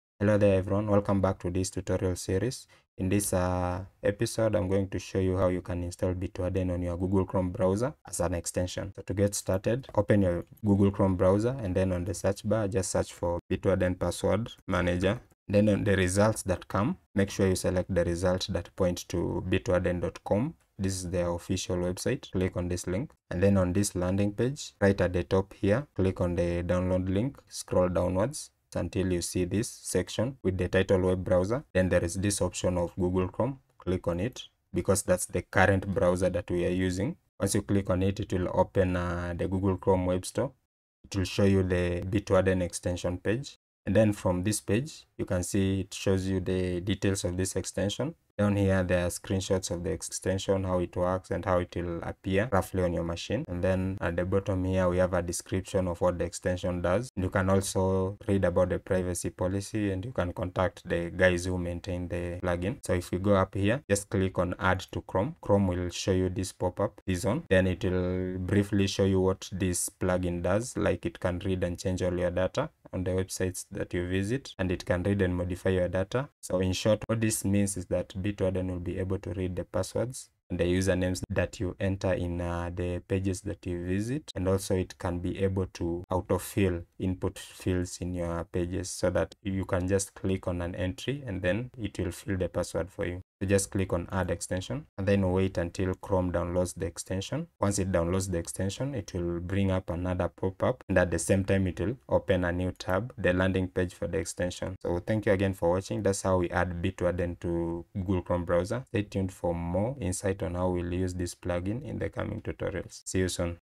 02. How to Install Bitwarden on Google Chrome
In this episode, I am going to show you how to install Bitwarden extension on your Google Chrome browser.